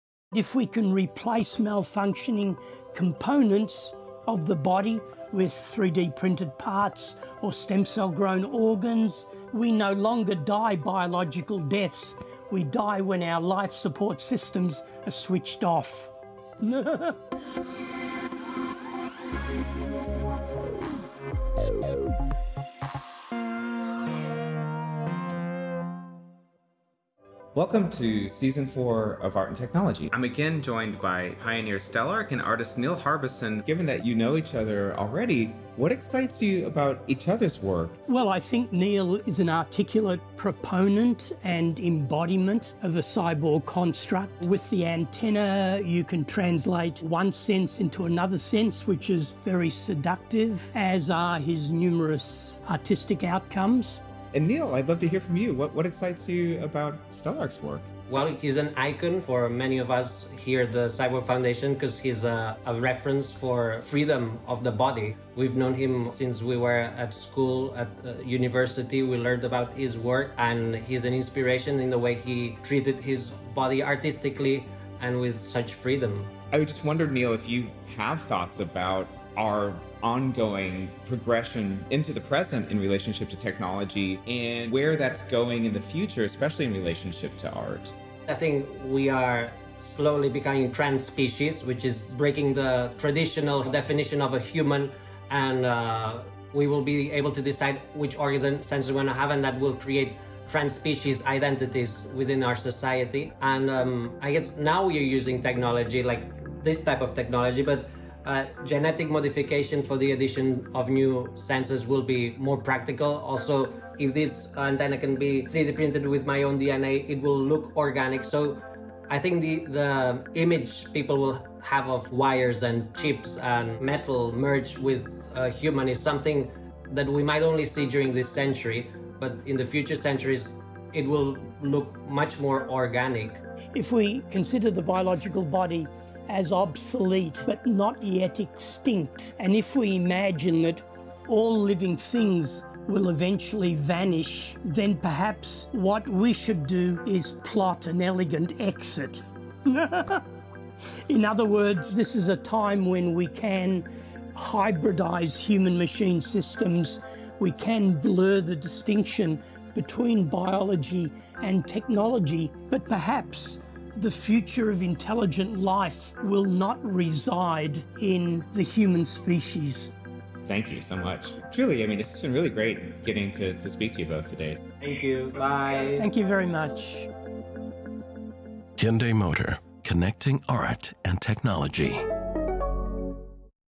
Season 4 Ep 11 - Part 2: DIRECTING OUR OWN EVOLUTION THROUGH ROBOTICS?
EPISODE 11: DIRECTING OUR OWN EVOLUTION THROUGH ROBOTICS?
PART 2
With stem-cell technology and space travel rapidly advancing, and viral agents prompting the global recognition that we are not the only intelligent lifeform out there, for Pioneer Stelarc and cyborg artist Neil Harbisson the time has come to embrace the inevitable demise of the human as is. Will we embrace a transpecies reality? Merging with mice, mycelium and more? Or perhaps we’ll start growing our own replacement organs from home. For these artists, treating the body with more freedom is the only sure way to survive the transition into the next century. Will you join them?
Stelarc artwork courtesy of the artist.
Neil Harbisson artwork courtesy of the artist and Cyborg Arts.